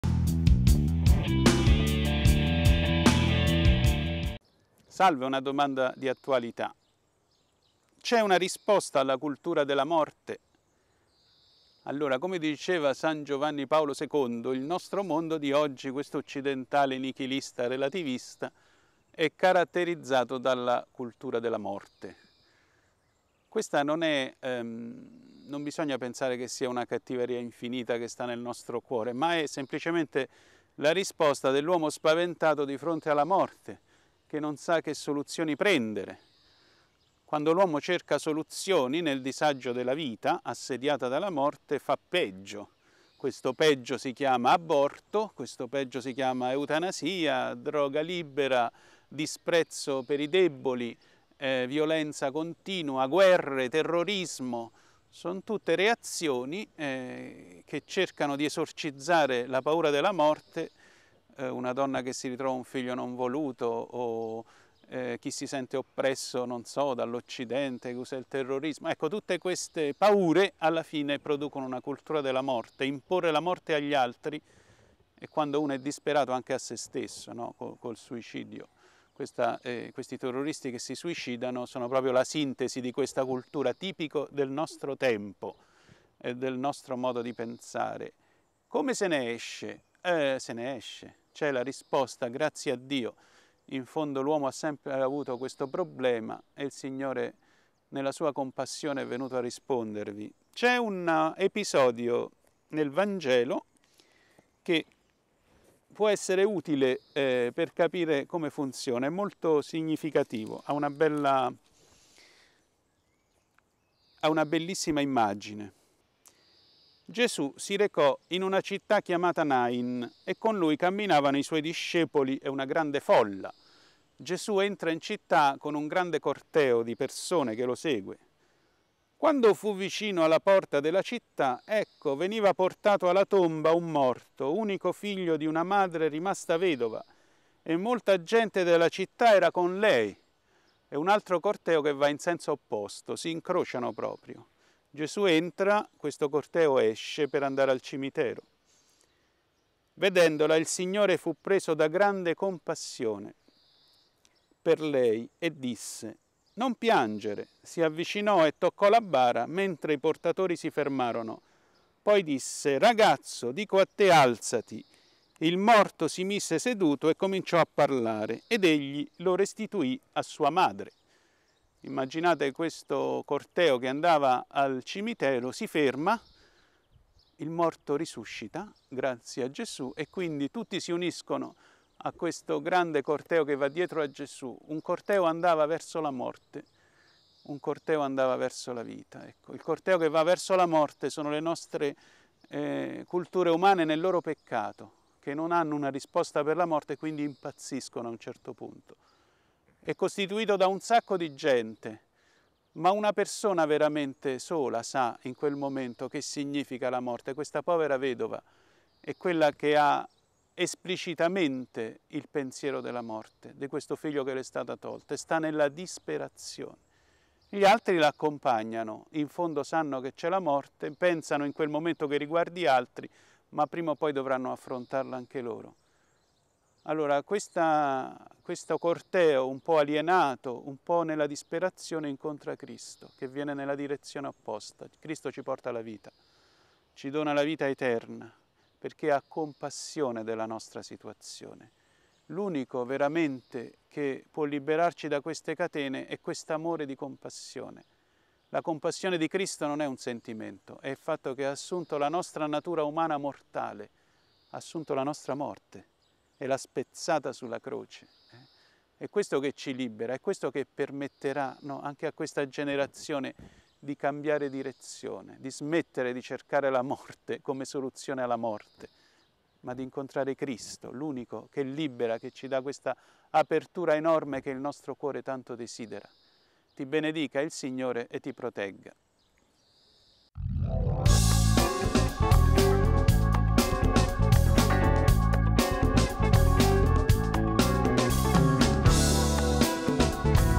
C’è una risposta alla cultura della morte?
Il Vangelo del giorno 05/06/16 risponde a una domanda della nostra vita quotidiana. Oggi chi ci propone una risposta è Don Antonio Grappone. Questa risposta ci aiuta ad andare oltre le domande.